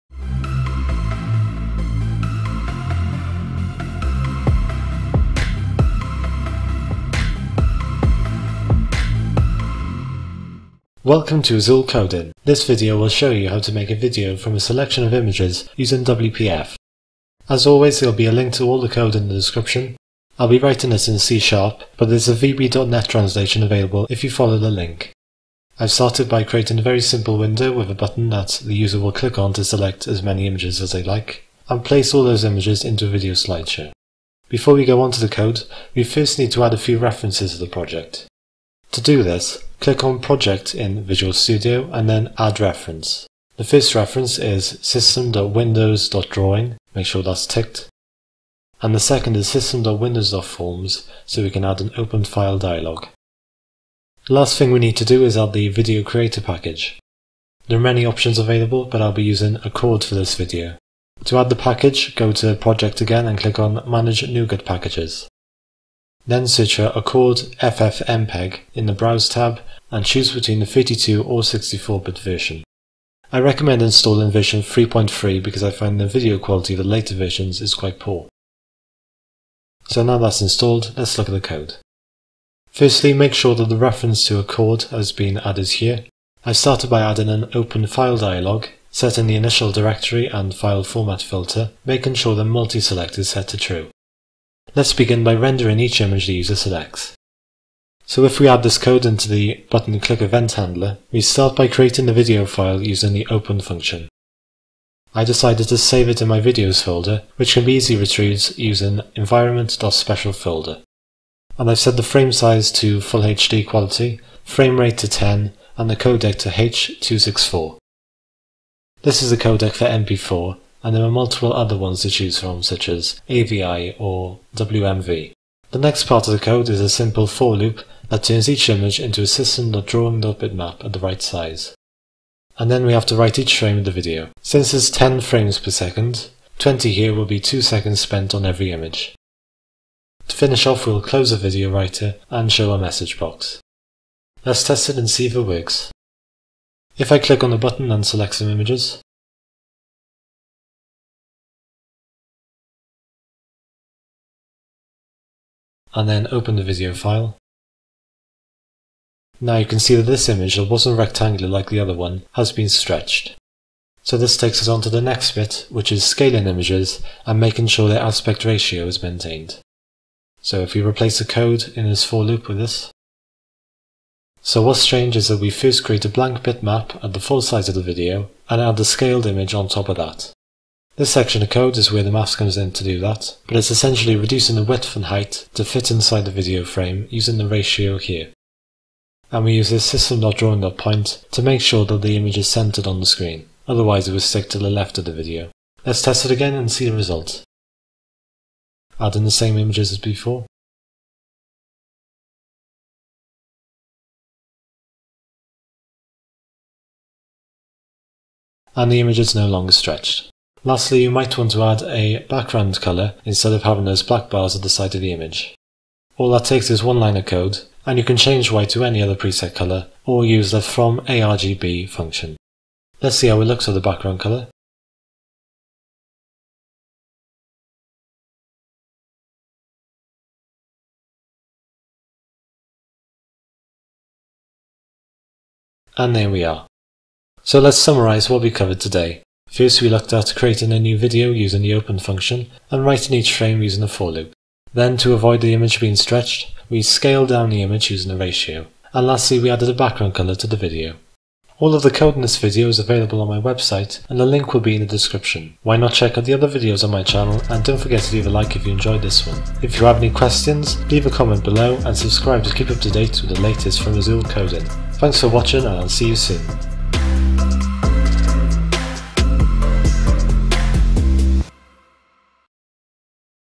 WPF C#/VB - Making a Slideshow Video from a Set of Images
This video will show you how to make a slideshow video from a selection of images using WPF. The code is available in C# and VB.NET here:

00:00 Intro
01:33 Part 1: Image Rendering
02:45 Part 2: Scaling Images
03:30 Part 3: Background Colour
03:57 Summary